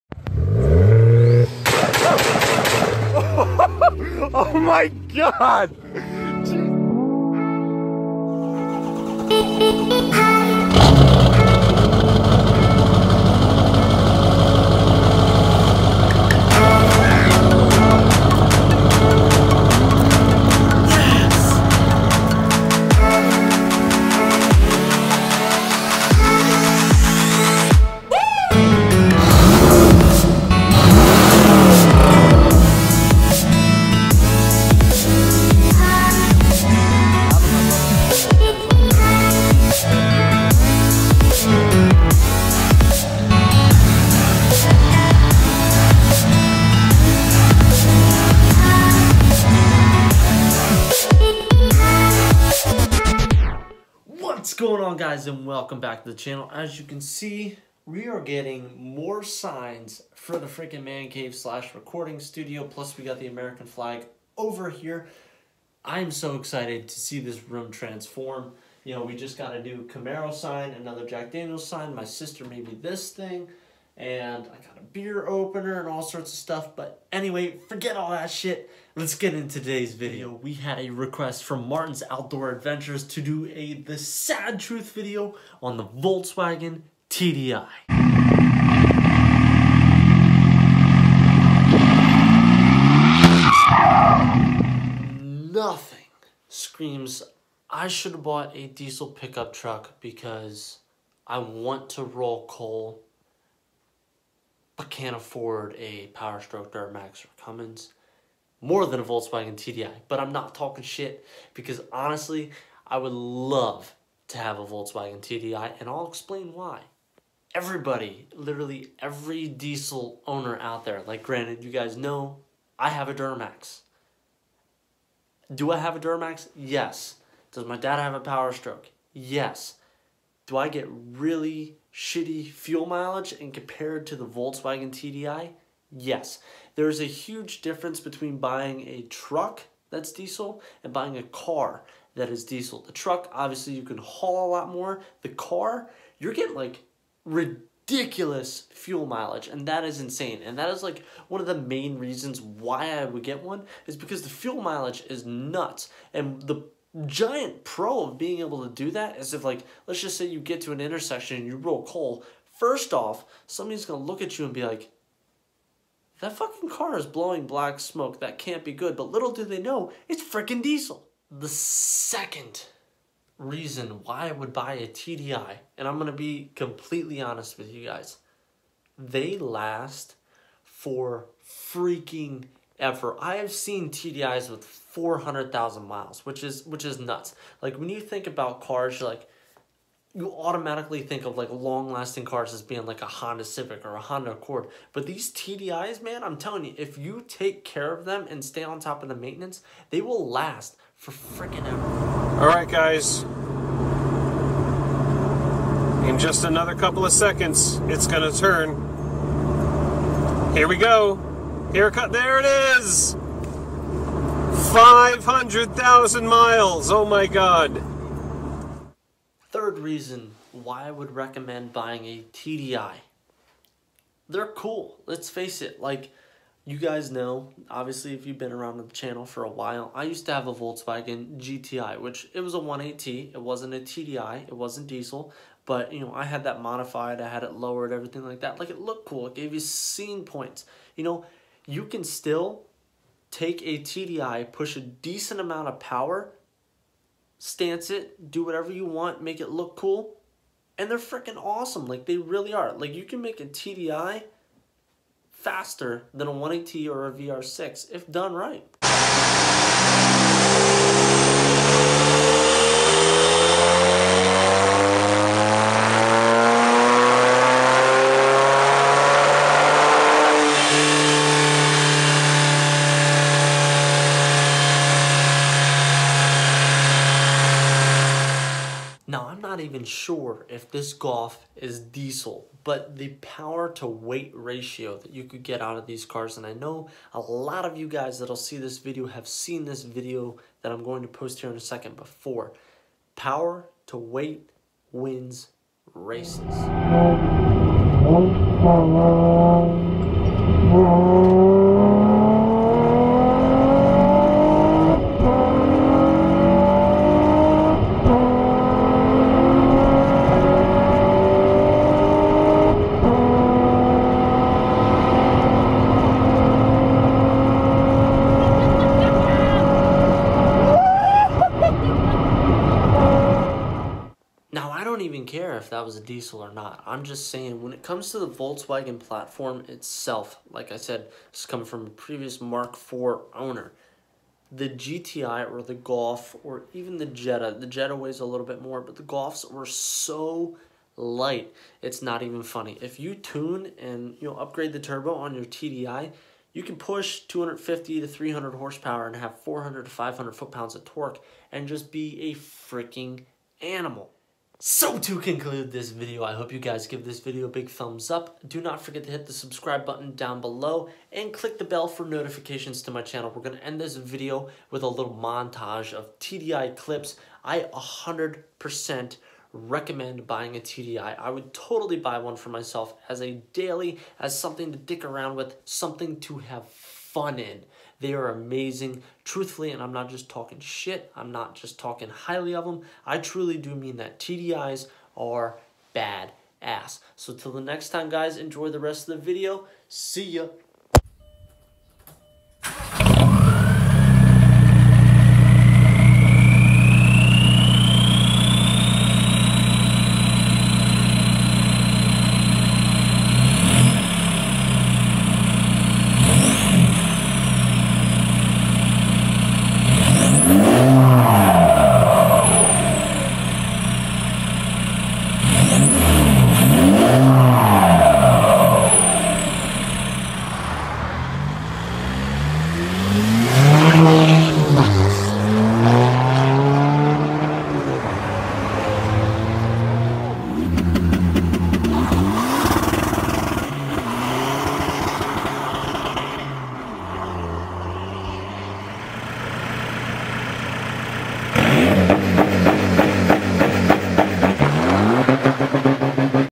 Buying a VW TDI?!? (The Sad Truth of the VW TDI)
today by request we are doing a video on the VW TDI and what my thoughts and opinions are!